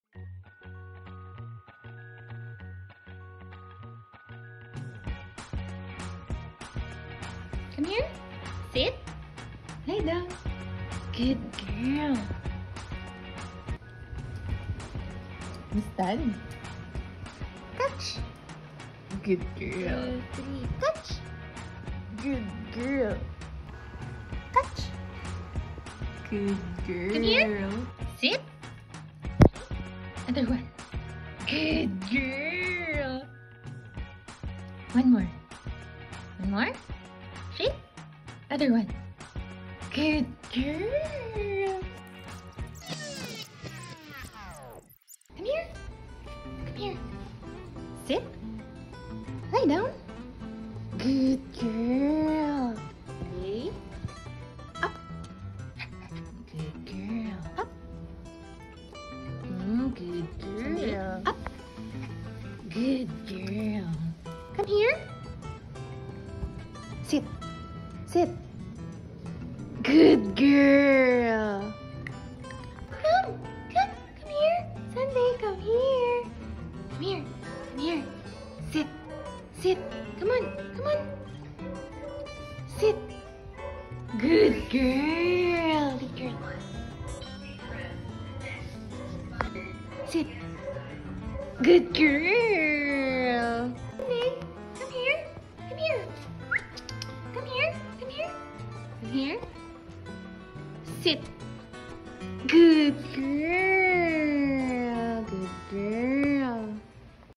Westie Dog Tricks
Sundae, the one-side floppy eared Westie, do tricks for treatos.

“Come here”
“Sit”
“Lie down”
“Up”
“Catch”
“Shake”